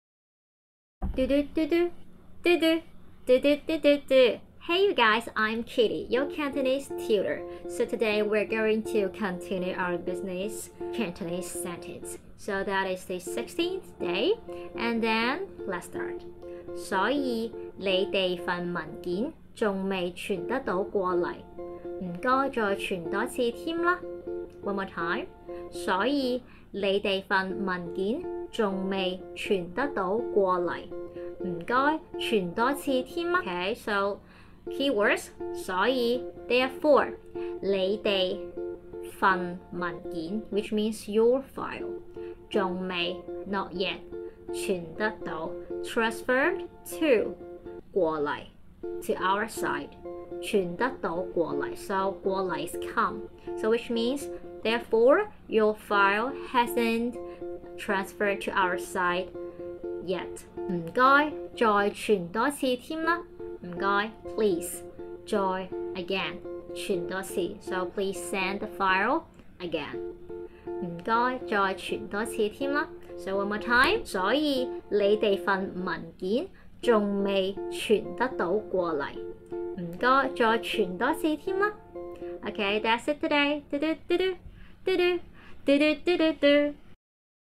Business Cantonse Day16
Episode 16|Oopsies! 🙈 Looks like I'll have to resend that file again - but hey, with all this practice, I'll be speaking fluent Cantonese in no time! 💪🏼💬

[ Learn Cantonese with Sunshine KY ]
Tag somebody who would do this.